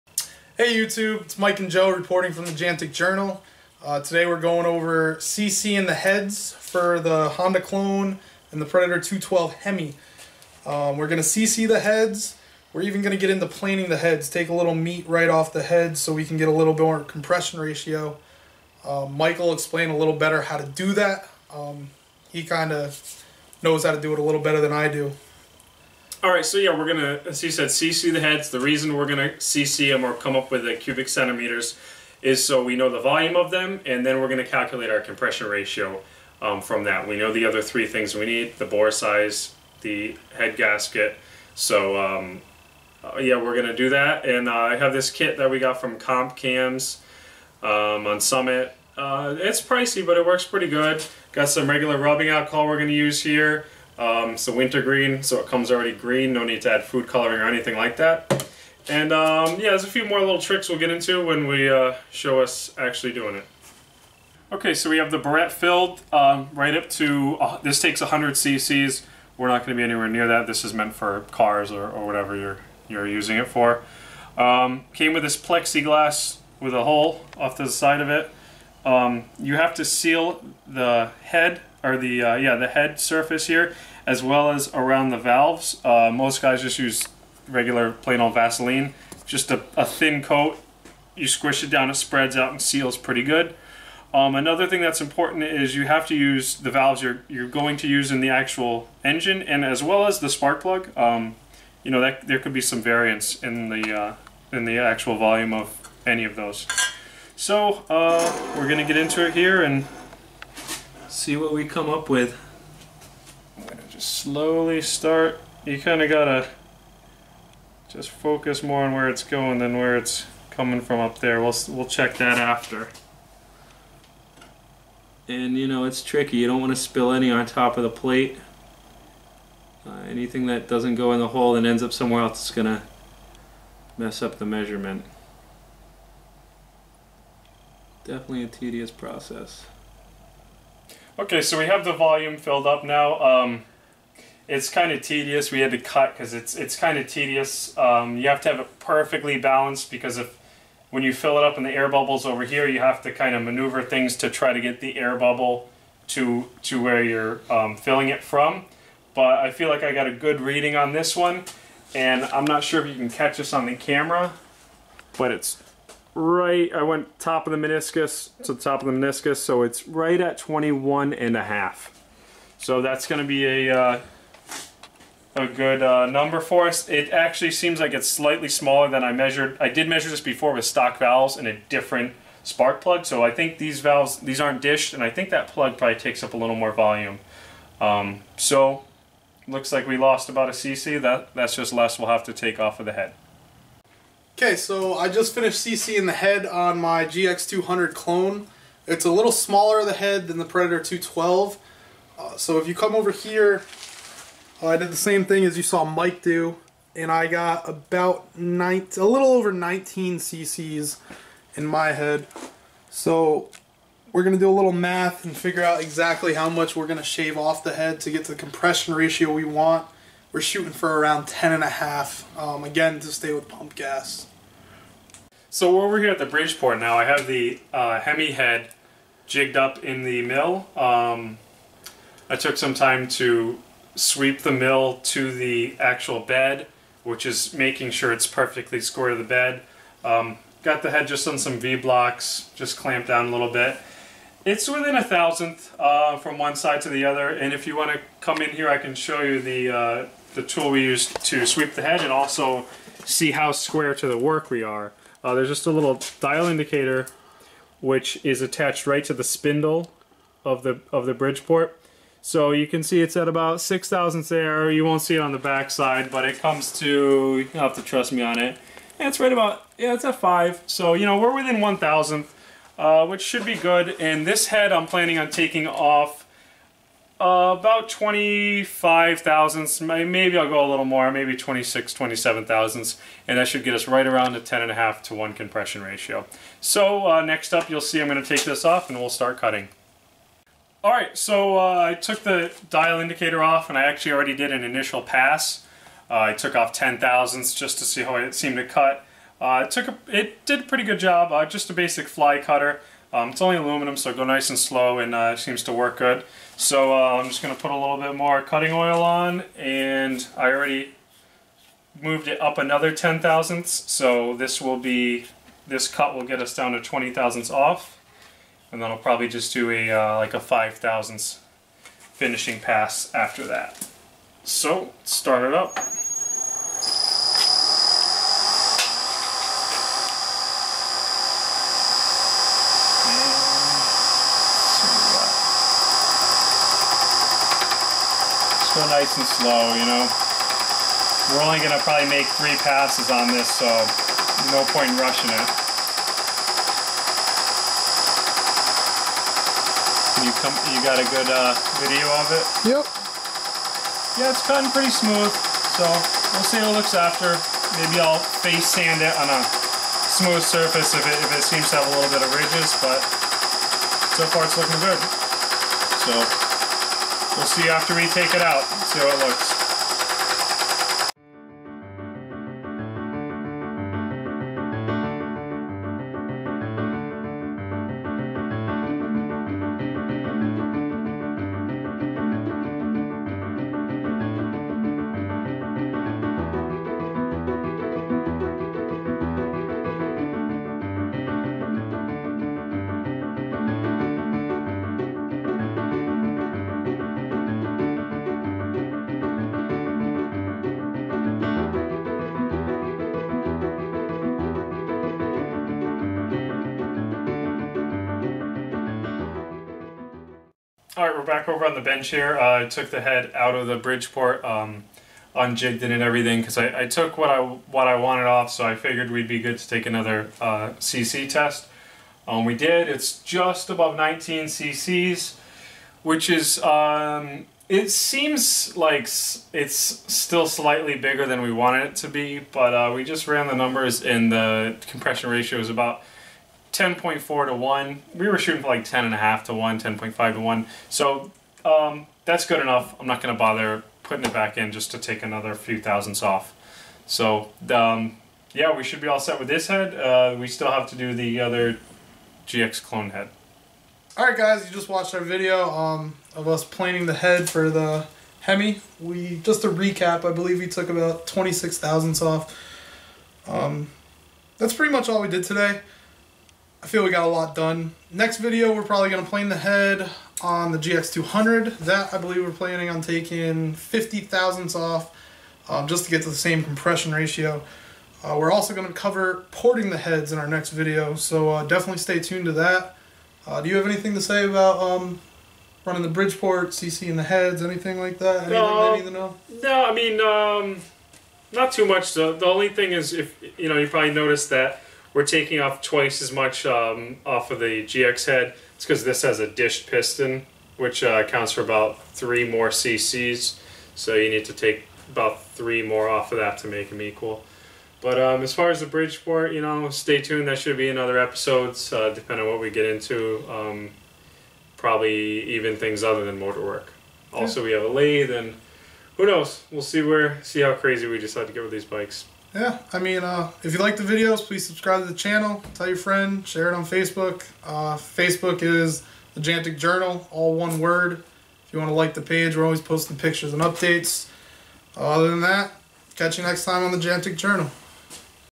CC test and Bridgeport milling Predator 212 hemi head
Today at Thejanticjournal we did a cc test on the predator 212 hemi head and gx 200 clone head. We're shooting for around a 10.5:1 compression ratio so that will require material to be planed off the heads and after figuring out the volume on our stock heads we then had a target volume to attain the desired compression ratio.